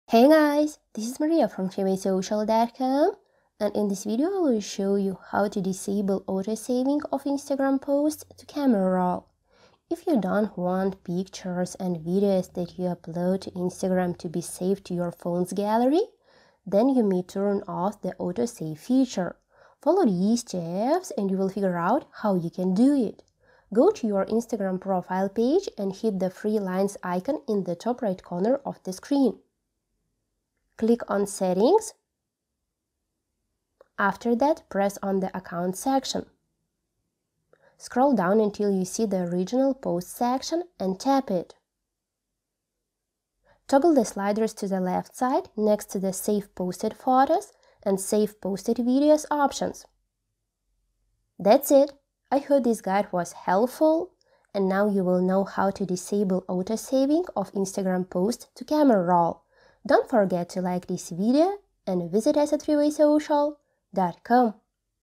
How To Disable Autosaving Of Instagram Posts To Camera Roll
In this video I am going to show you how to disable autosaving of Instagram posts to camera roll.

If you don’t want pictures and videos that you upload to Instagram to be saved to your phone’s gallery, then you may turn off the autosave feature.

Follow these steps and you will figure out how you can do it:

1. Go to your Instagram profile page and hit the three lines icon in the top-right corner of the screen.
2. Click on Settings.
3. After that press on the Account section.
4. Scroll down until you see the Original Pots section and tap it.
5. Toggle the sliders to the left side next to the save posted photos and save posted videos options.

That’s it!

I hope this guide was helpful and now you will know how to disable autosaving of Instagram posts to camera roll. Don’t forget to like this video and visit us